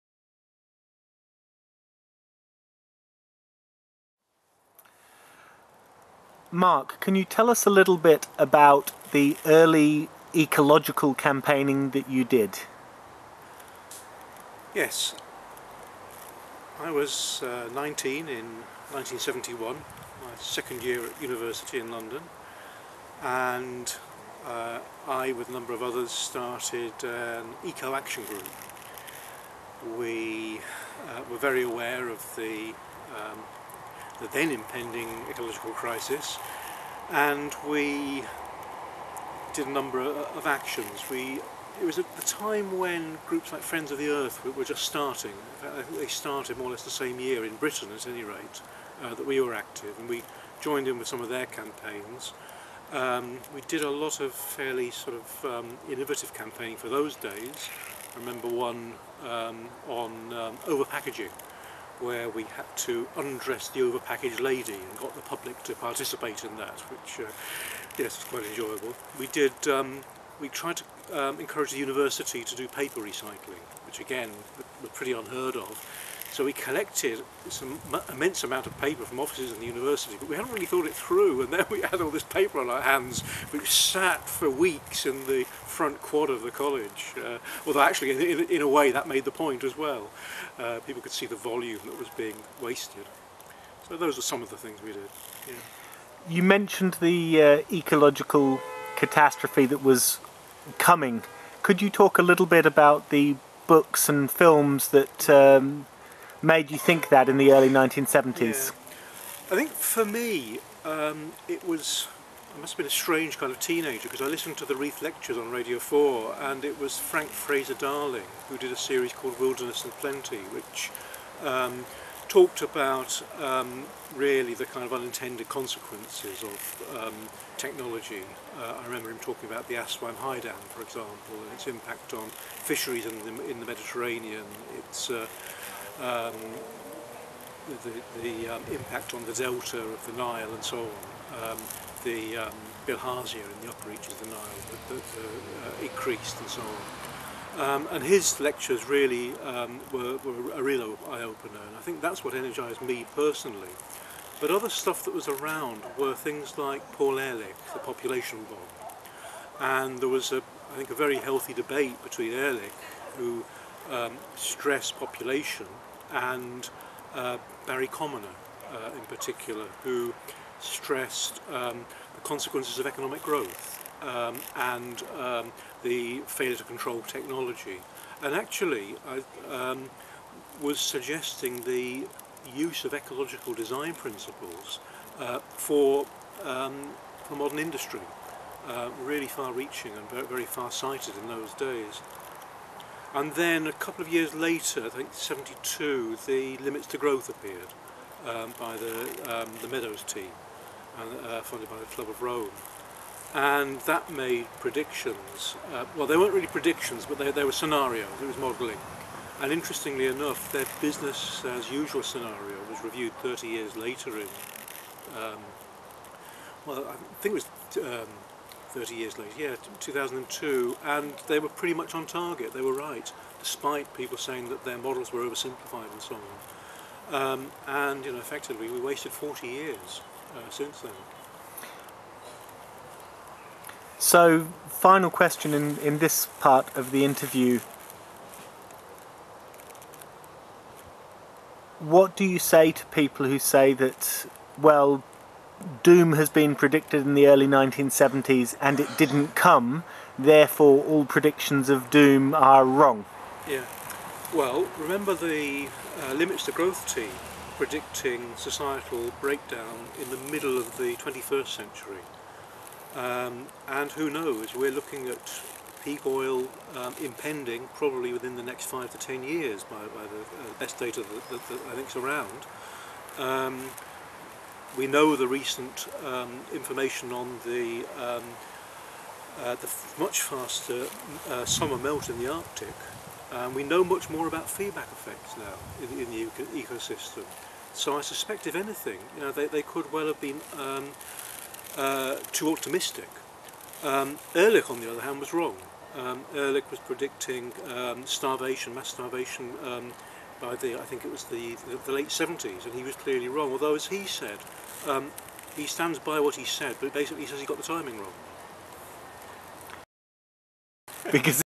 "1970s and now" Mark Burton interview
Discussion of the early days of "ecological action", what inspired it, what the successes and mis-steps of it were.